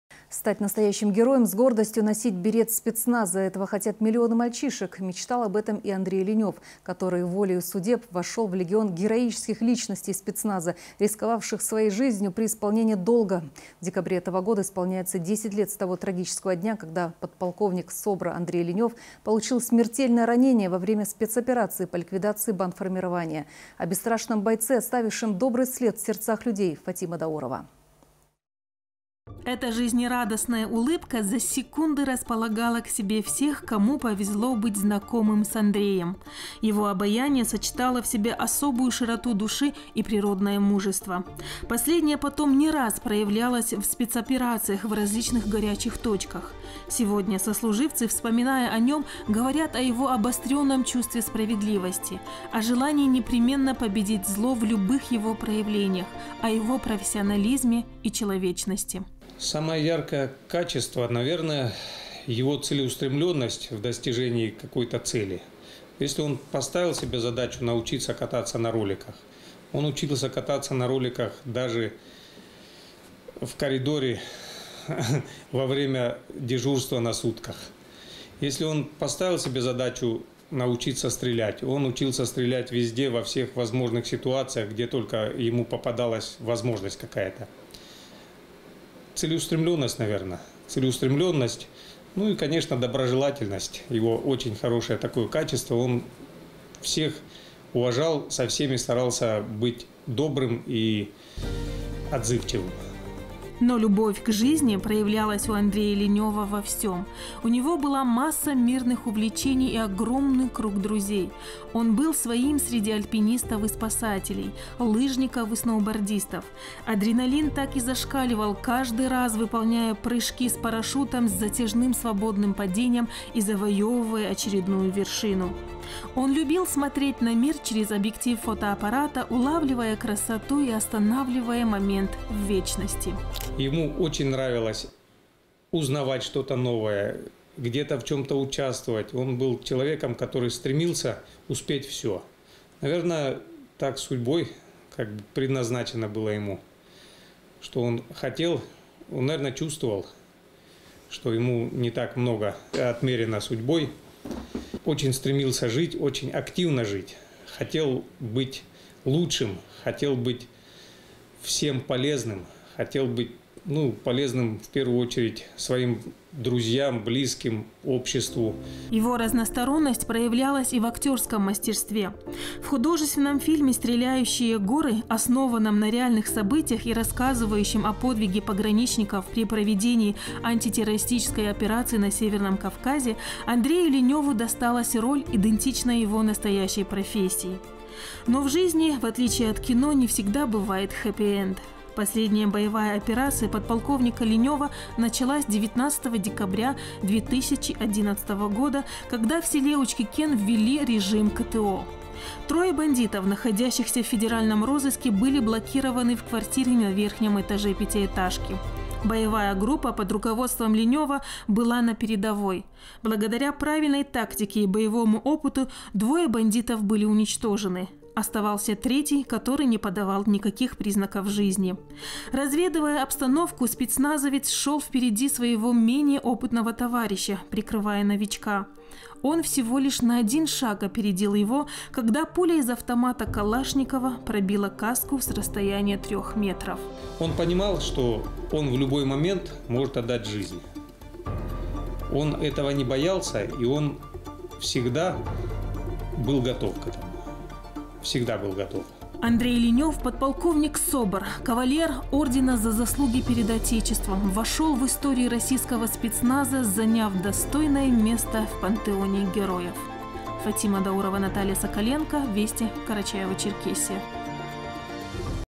Герои среди нас
Стать настоящим героем, с гордостью носить берет спецназа - об этом мечтают миллионы мальчишек. Мечтал об этом и Андрей Ленев, который волею судеб вошел в легион героических личностей спецназа, рисковавших своей жизнью при исполнении своего долга.  В декабре этого исполняется 10 лет с того трагического дня, когда подполковник СОБРа Андрей Ленев получил смертельное ранение во время спецоперации по ликвидации бандформирования